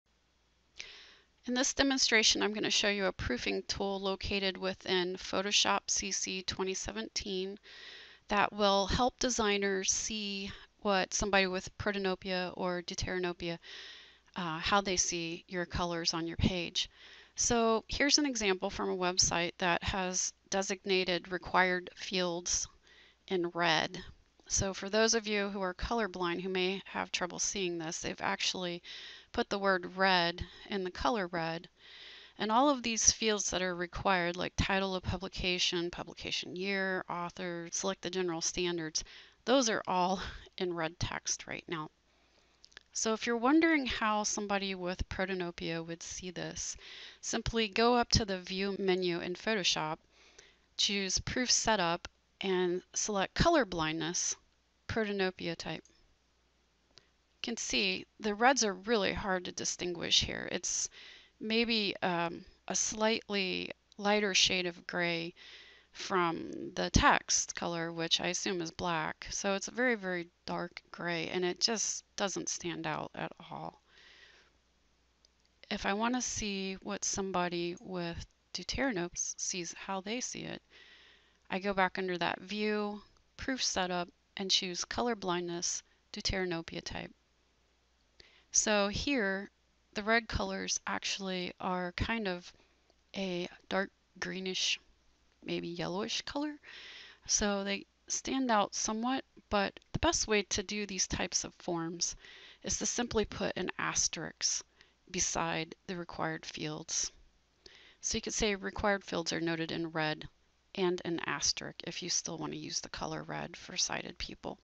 Photoshop Colorblindness Simulation Proof Tool
An introduction to Photoshop's color proofing tools to simulation Protanopia and Deuteranopia.